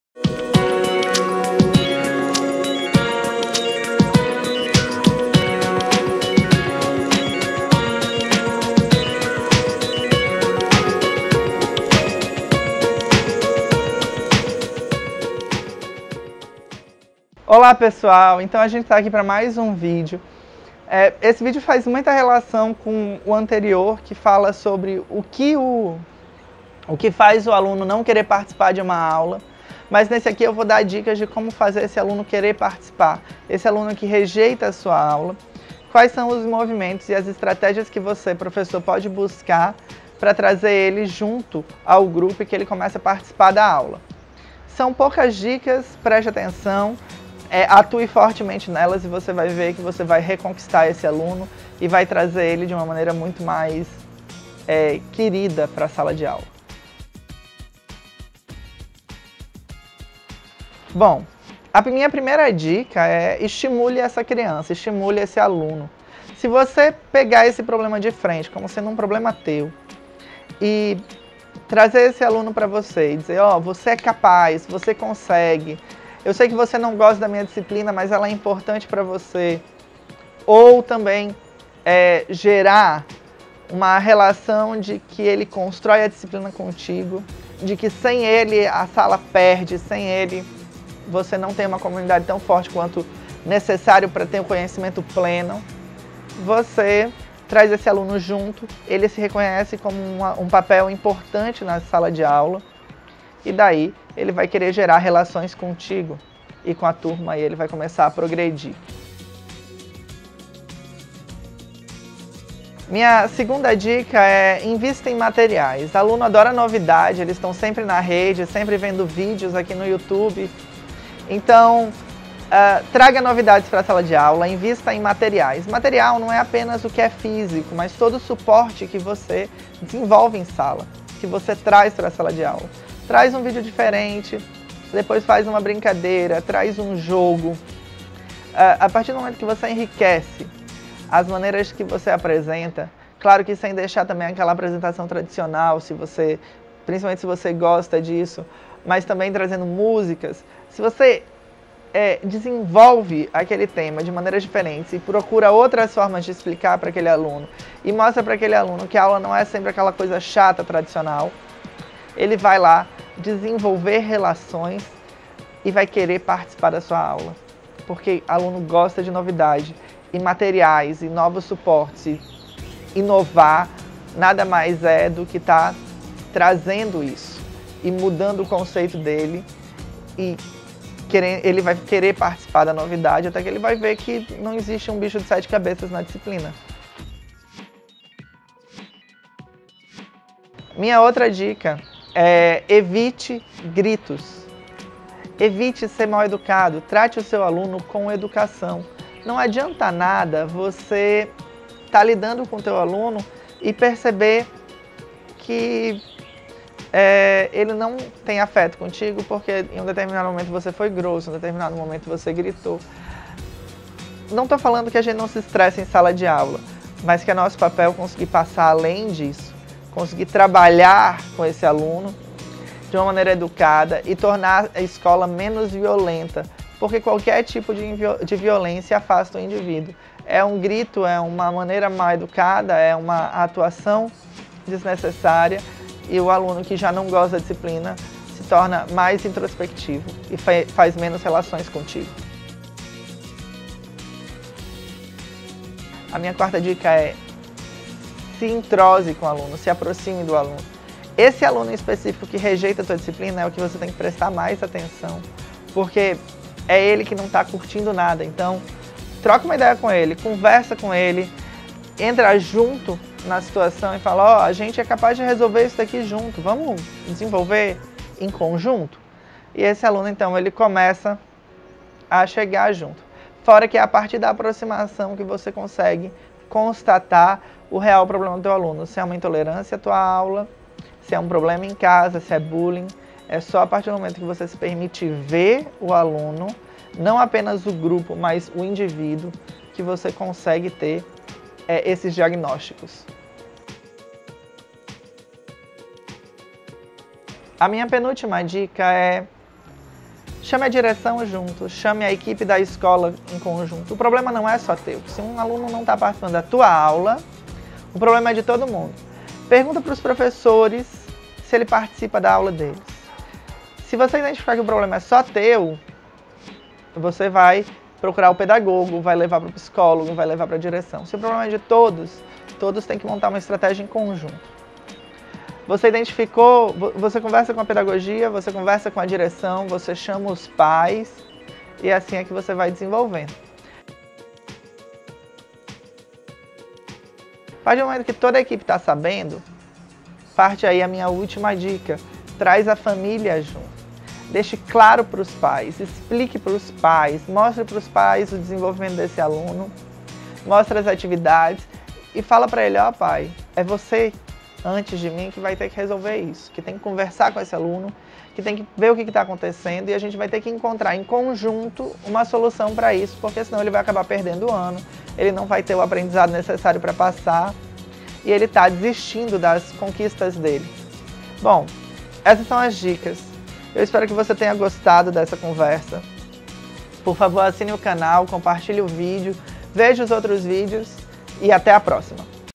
Como lidar com alunos rebeldes?
Neste vídeo apresento estratégias para lidar com alunos rebeldes que se negam a participar da aula e fazer as atividades.